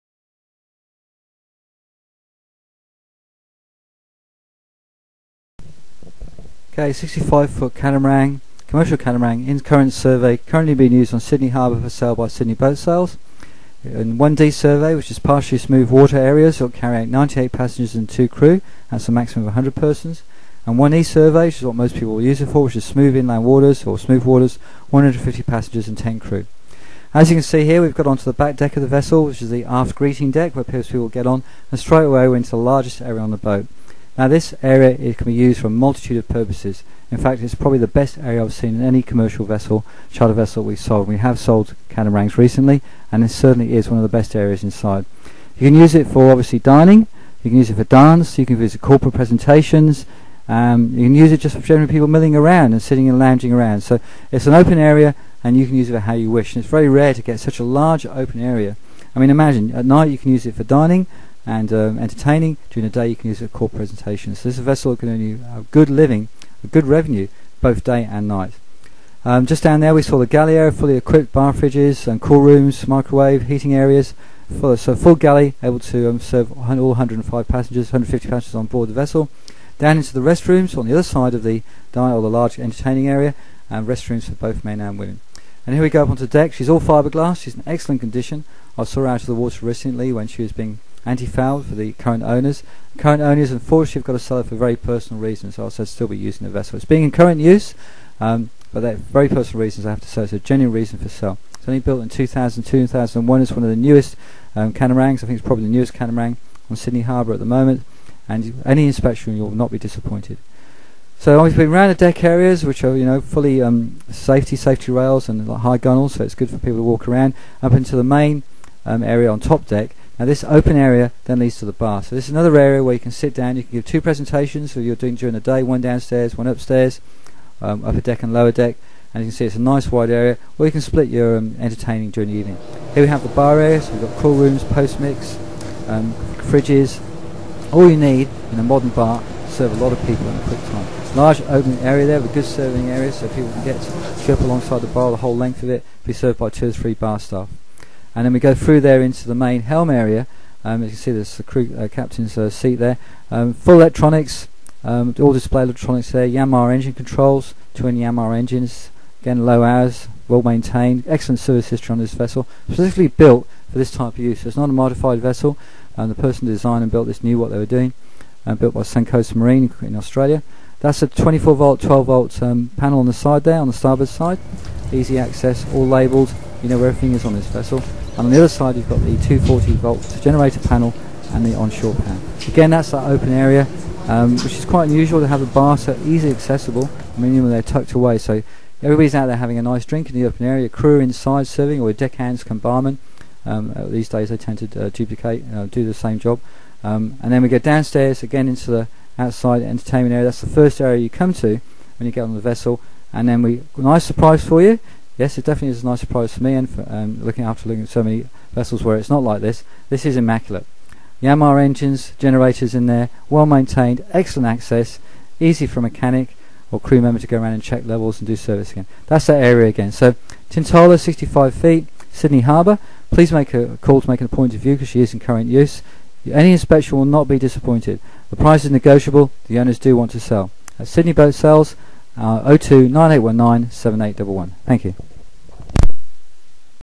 65' Commercial Catamaran Sydney Boat Sales
This luxury Sydney Based Charter vessel is available for immediate sale "Tintola" was first launched in November 2005 and is one of the newest & most luxurious charter yachts currently in use on Sydney Harbour. She was designed & built for her current owner an experienced charter operator who wanted to offer guest's a superior cruising experience. Her 29 ft beam makes her extremely stable with vast indoor & outdoor entertaining areas on 2 levels offering spectacular 360 degree views. With a functional & versatile layout she lends herself to a multitude of uses whether it be corporate entertaining, dinner cruisers, meetings,weddings the only limit is your imagination. Priced well below replacement cost "Tintola" is seriously for sale ideal for those wanting to enter the lucrative large charter boat market or those wanting to add a flagship to their existing fleet. In Current Survey 1D for 98 Passengers + 2 crew in partially smooth waters & 1E survey for 150 passengers + 10 crew in smooth waters. Lying on a berth in Sydney 5mins by boat from our office please call to arrange an inspection. Open Mon-Sat by appointment & Sundays depending on availability.